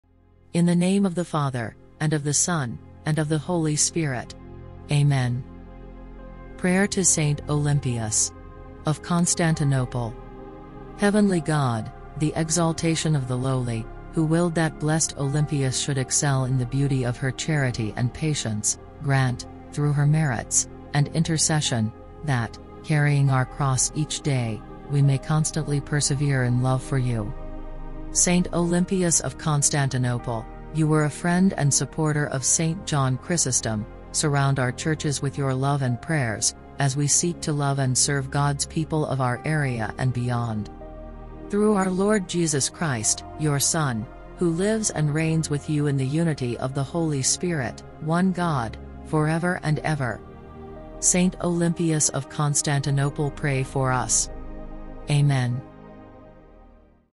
Prayer to Saint Olympias - Deaconess of Constantinople
Olympias was born about 361 into a wealthy family in Constantinople. She married a rich prefect but was widowed at an early age. She never re-married, choosing instead to live in community with other women. The community lived close to John Chrysostom’s Patriachal church in Constantinople. Under Olympias’  leadership the women dedicated themselves to hospitality, generosity to the poor, and care for the sick. She went on to extend her care for those on the margins by establishing an orphanage and a hospital.

Olympias was a firm supporter of John Chrysostom. When Chrysostom was sent into exile Olympias continued to stand up for him, and was herself sent into exile in  404 (for a note on that date relating to web pages see comments below!). In this exile she suffered depression and other illnesses, but was comforted by her correspondence with John Chrysostom.  She died in exile, less than a year after the death of Chrysostom. (Feast day December 17th).

Faith and works should never be far apart. Olympias is a great inspiration for us all, and especially for us at St Chrysostom’s. She reminds us of the importance of hospitality and generosity, care for the marginalised and courage in the face of adversity.